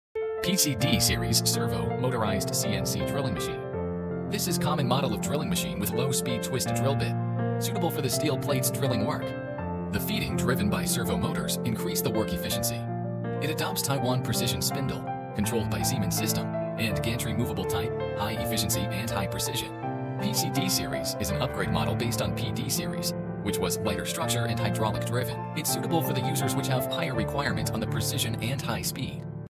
High Quality Raintec cnc planar drilling machine
The machine is mainly used for drilling and milling of steel structure,irontower,bridge,mechanical processing and other industries,such as connectingplate and floor board,drilling and milling of tubes,baffles and flanges inindustries such as boiler petrochemical,pressure vessel and so on; drillingholes,blind holes,step holes,chamfering and light milling.